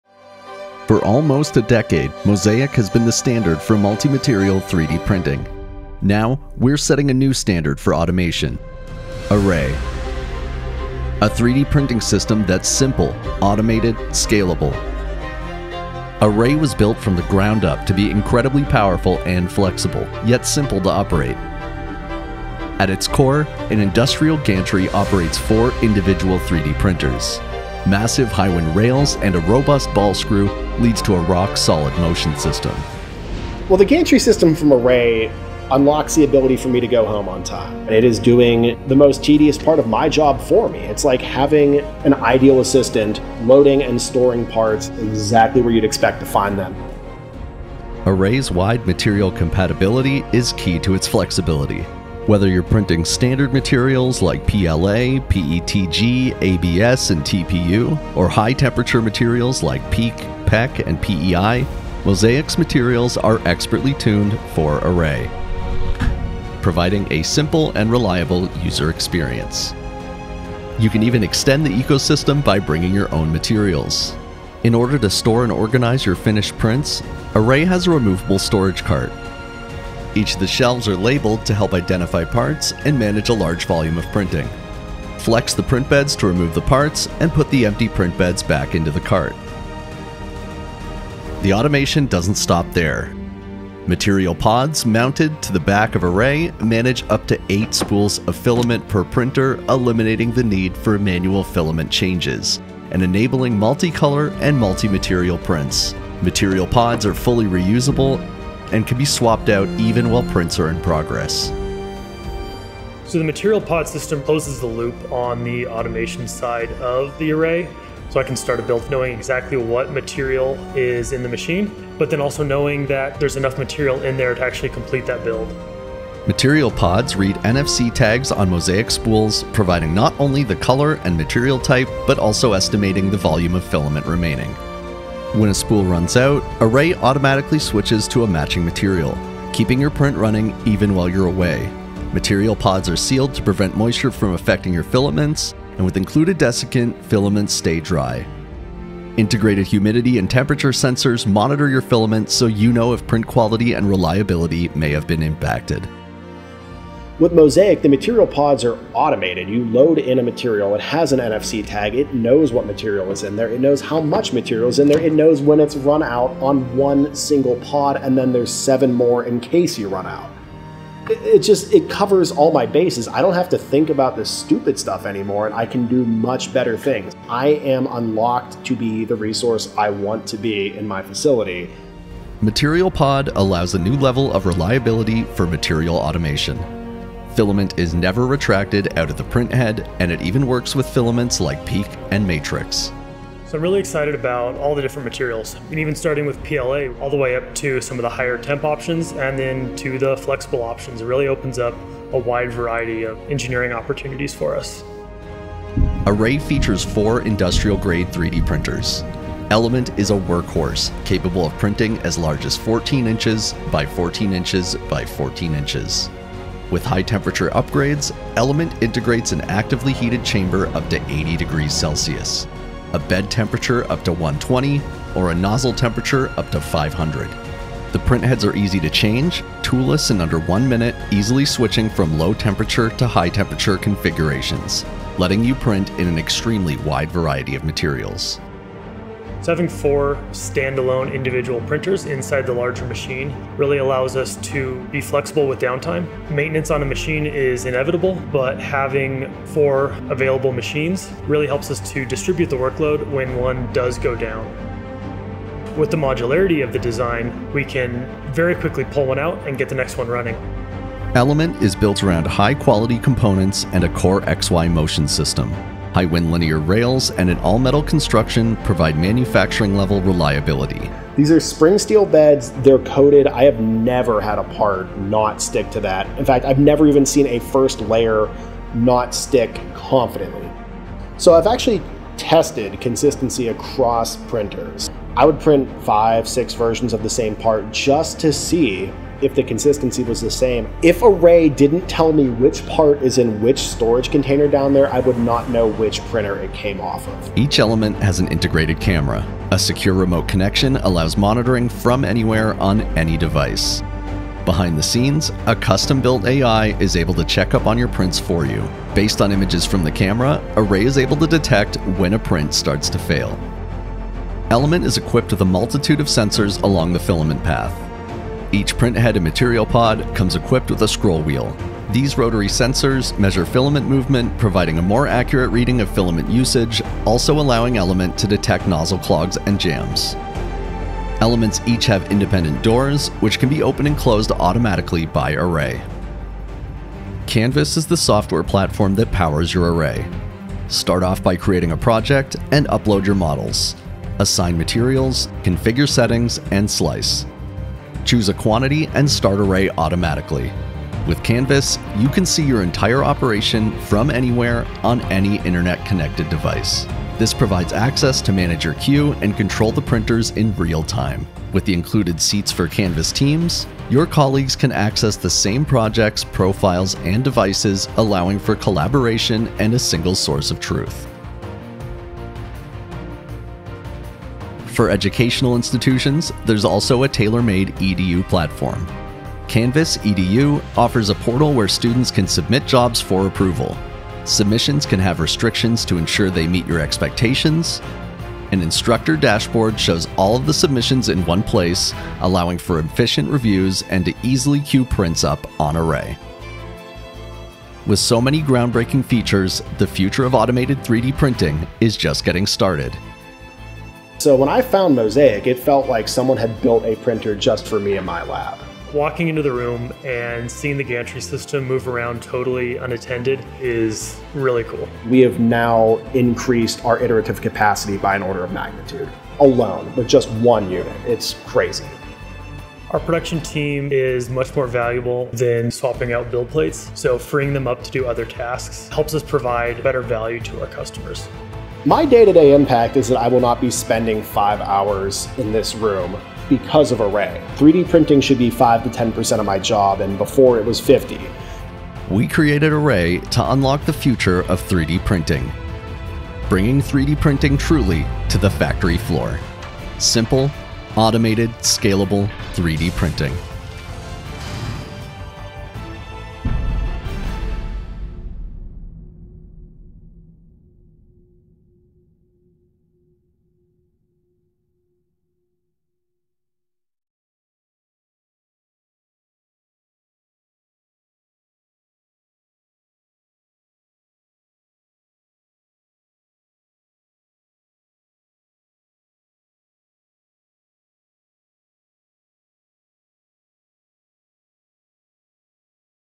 Array: The Future of 3D Printing | Mosaic Mfg.
Unlock automated, high throughput and cost effective Fused-Filament Fabrication printing with Array.

Array is 4 industrial grade printers with high volume printing supported by a robotic gantry and storage cart that allows for over 72 hours of uninterrupted printing. Each printer has either a 300C print head or a 500C print head, along side a heated chamber and print bed to support printing with industrial materials like PEEK, PEKK and PEI 9085.

Coupling this system with Mosaic Material Pods unlocks printing with up to 32 spools of filament, for unrivalled machine up time. Pairing that with 14"x14"x14" build volumes on each of the four printers makes the dream of high-volume additive manufacturing, a reality.

Attributions for models:

M250 Moskito Racer Drone by Hauntemonium